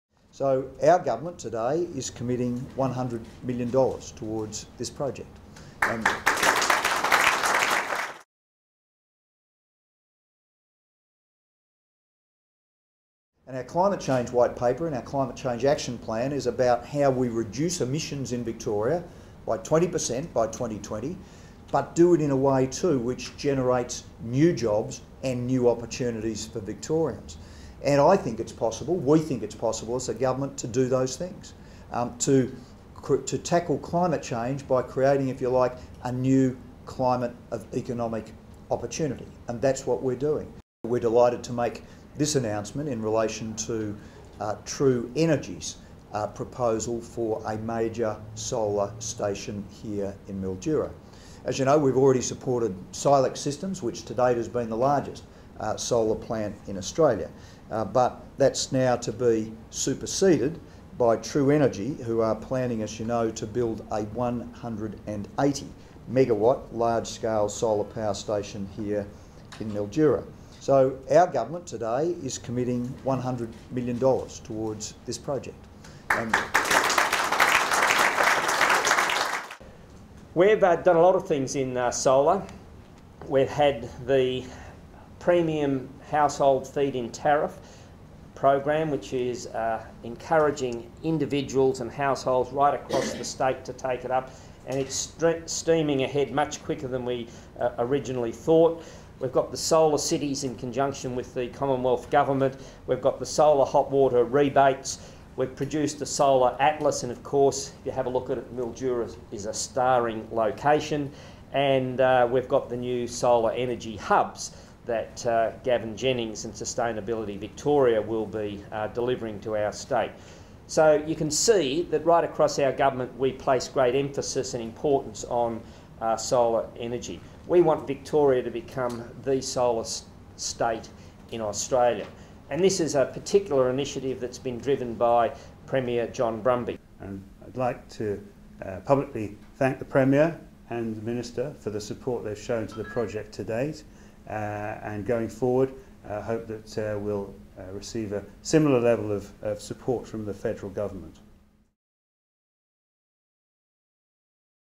John Brumby - Solar power station to be built in Victoria's north-west
Victoria is set to have the largest solar power station in Australia with the Victorian Government committing $100 million to a new large-scale solar development in the State's north-west.

In Mildura, Mr Brumby announced that the Victorian Government would back a bid by energy company TRUenergy to develop a major new solar plant south of Mildura with $100 million for renewable energy generated.

Mr Brumby said TRUenergy's proposal to build the Mallee Solar Park would create 200 new jobs in construction and 20 ongoing jobs, deliver 345 gigawatt hours of clean electricity each year and generate enough power to run 60,000 homes each year.

"Together with Victorians, we are rising to the challenge of climate change by driving new initiatives to cut emissions and create a cleaner, greener future for Victorian families," Mr Brumby said.

"Our Government's disciplined economic management means Victoria is in a position to tackle climate change by backing new investments in renewable energy to lock-in a sustainable future.

"This investment is about capitalising on Victoria's potential to generate renewable energy and forms a key part of our commitment to increase Victoria's electricity supply from large-scale solar power to five per cent by 2020.

"The TRUenergy proposal will utilise world's best-practice to create jobs in regional Victoria, cement the northern part of our State as a key solar energy region in Australia and will make a valuable and lasting contribution to our clean, green energy supply."

The Government set aside $100 million in funding to support a large-scale solar development last year and it forms a part of its Climate Change White Paper Action Plan, released in July.

Under the TRUenergy's proposal a photovoltaic plant will be built on a 600 hectare site about 10 kilometres south of Mildura with construction to be undertaken by Bovis Lend Lease in four stages between 2012 and 2015.

"TRUenergy's project is a great example of the significant jobs and investment expected across regional Victoria as a result of our White Paper which will drive new investment of between $6 and $10 billion," Mr Brumby said.

"Through our Climate Change White Paper Action Plan we have committed to making Victoria Australia's solar state and aim to support the development of five to 10 large scale solar plants in regional Victoria by 2020.

"Under the Action Plan our Government has also committed to cut emissions by at least 20 per cent by 2020 and increase the state's power generated by solar to five per cent."

Minister for Energy and Resources Peter Batchelor said TRUenergy's proposal was selected through an independent tender from a strong field of five applicants after it submitted an outstanding proposal under the Victorian Large Scale Solar Project.

"This project will use the latest in solar photovoltaic technology, utilising flat-plate, thin film photovoltaic panels which will be made by First Solar, a US based company," Mr Batchelor said.

"It will build on Silex's large-scale demonstration photovoltaic solar project planned for near Mildura which is being supported by a $50 million Brumby Labor Government grant."

Mr Batchelor said with $100 million from the Victorian Government, the project proponents would become a stand-out applicant for Solar Flagships funding from the Gillard Government.

"We have backed this proposal because it has all the right characteristics to deliver major benefits to for communities, for the environment and for a cleaner, greener economy," he said.

"We believe it is now a stand-out for funding from the Commonwealth's Solar Flagship Program."

For more information about the Victorian Government's solar commitments visit the Office of Solar Energy